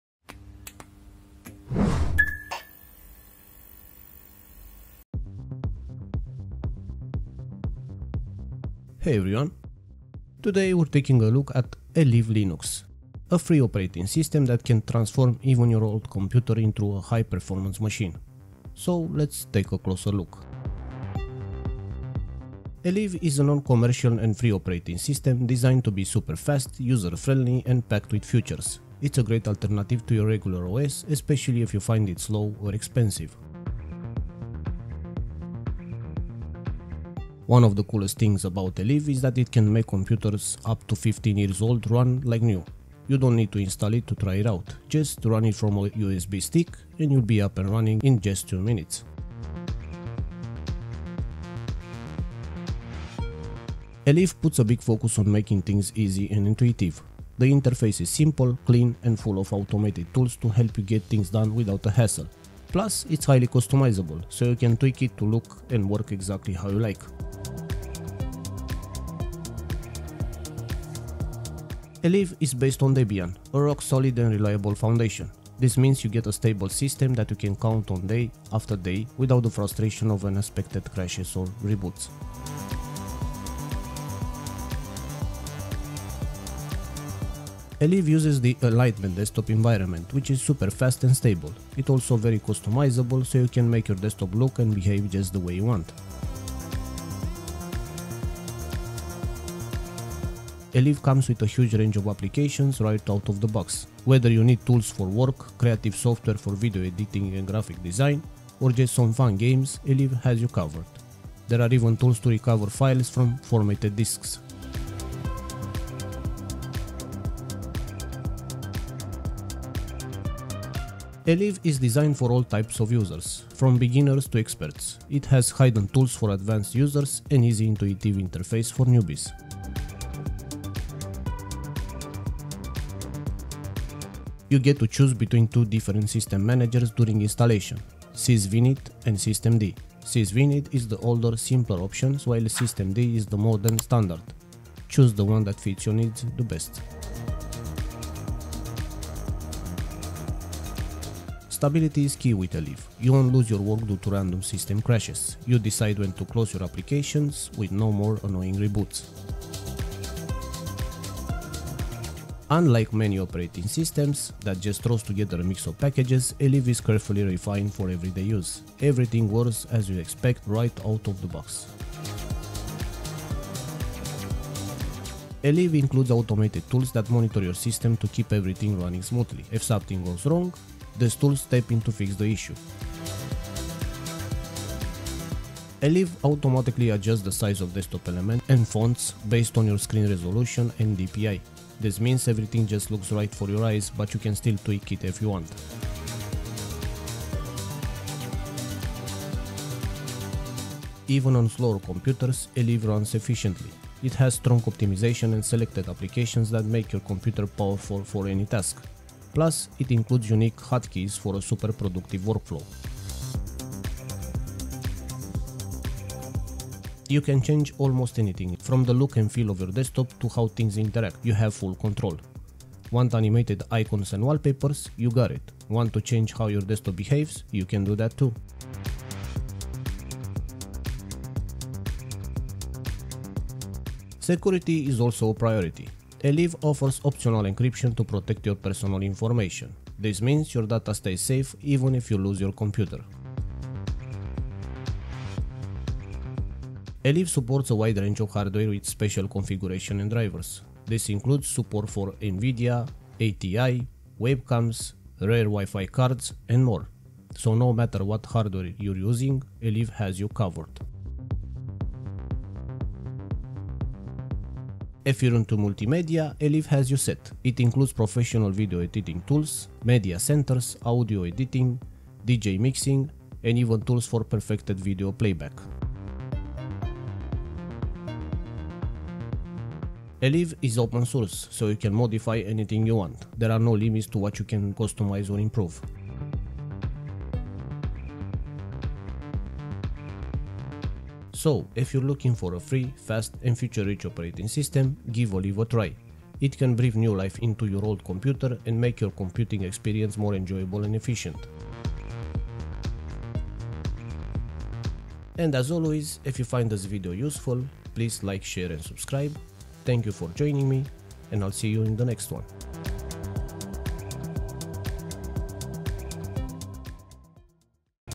Elive Linux 3.8.42 (beta)
Elive, or Enlightenment live CD, is a Debian-based desktop Linux distribution and live CD featuring the Enlightenment window manager. Besides being pre-configured and ready for daily desktop use, it also includes "Elpanel" - a control centre for easy system and desktop administration.